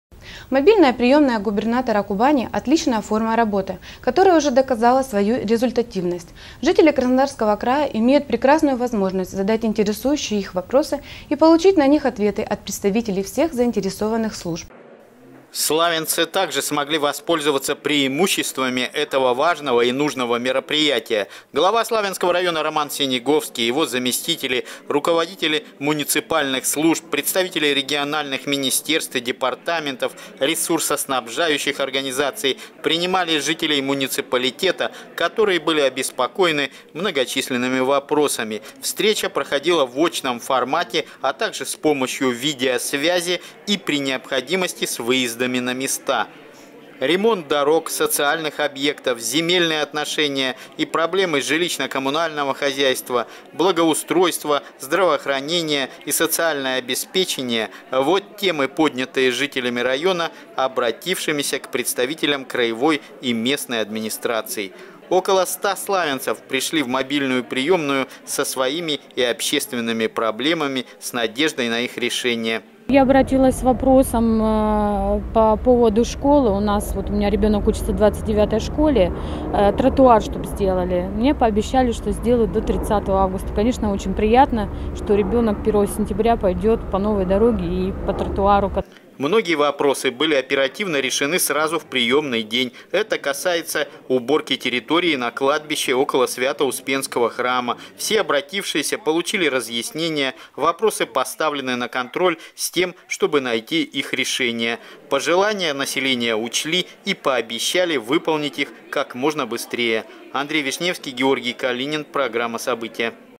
Мобильная приёмная губернатора работала в нашем районе
Мобильная приёмная губернатора Кубани – отличная форма работы, которая уже доказала свою результативность. Жители Краснодарского края имеют прекрасную возможность задать интересующие их вопросы и получить на них ответы от представителей всех заинтересованных служб